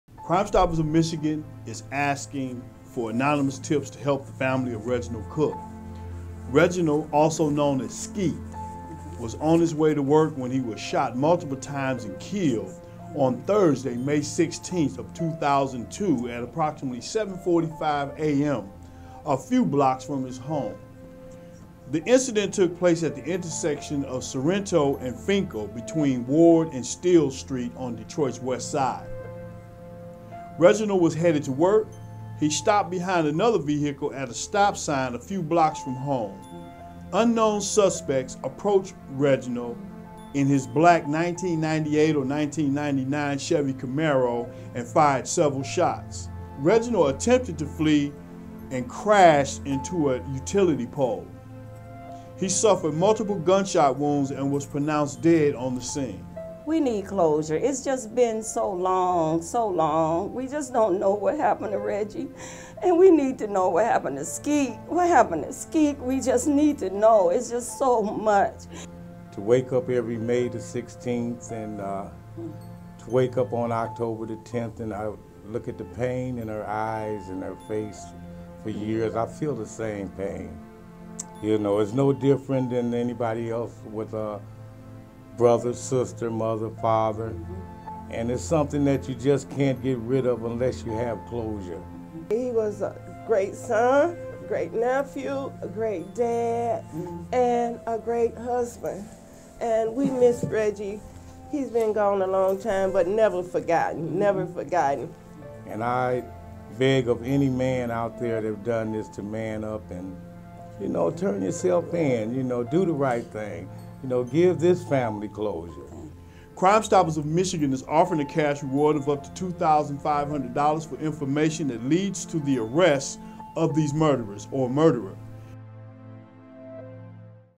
Reginald Cook press conference
Reginald Lamont Cook a.k.a. “Skeet” was murdered on, Thursday, May 16, 2002 at approximately 7:45 a.m. The incident took place at the intersection of Sorrento and Fenkell, between Ward Ave. and Steel St. on Detroit’s west side. Reginald was headed to work. He stopped behind another vehicle at a stop sign a few blocks from home. Unknown suspect(s) approached Reginald in his black 1998-99 Chevy Camaro and fired several shots. Reginald attempted to flee and crashed into a utility pole. He suffered multiple gunshot wounds and was pronounced dead on the scene.